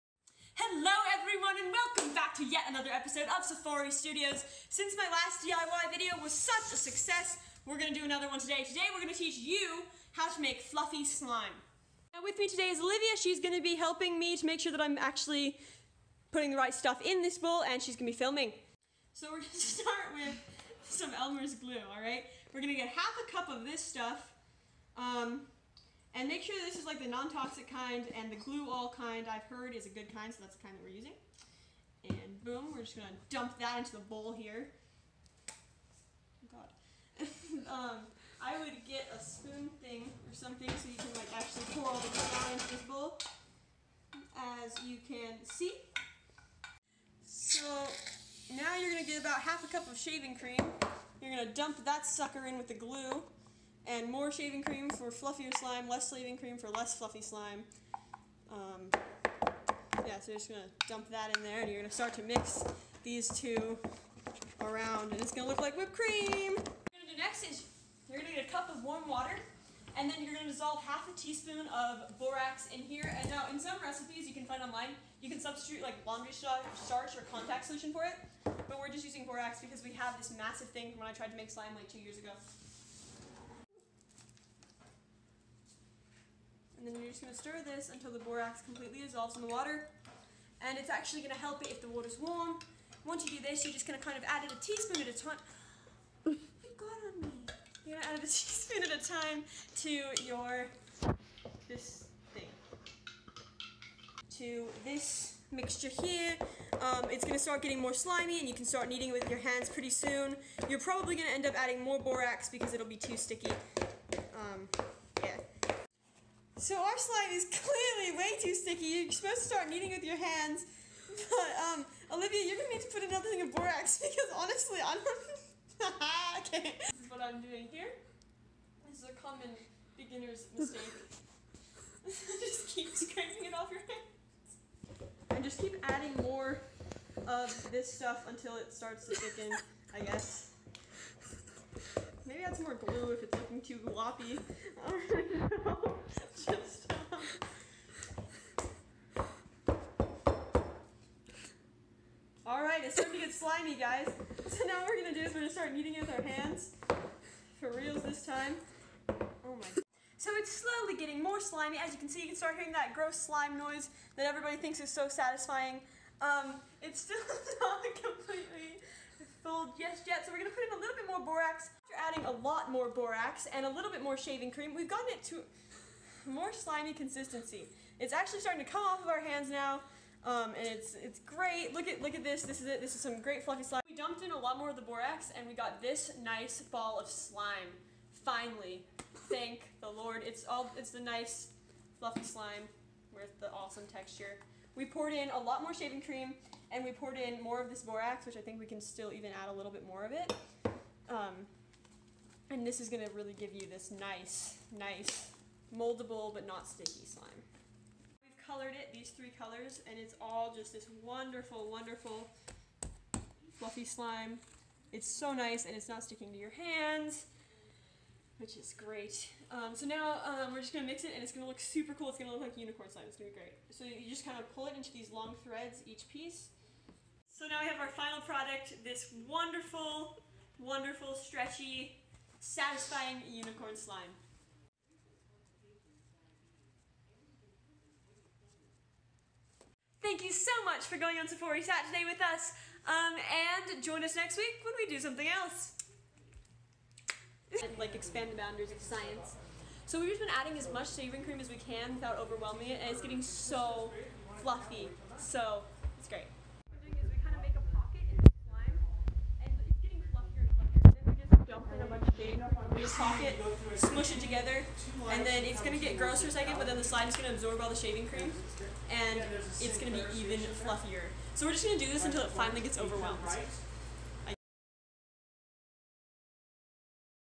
Easy DIY fluffy slime 3 ingredients!
This week on Adventures with Abi, I walk you through making FLUFFY SLIME!!!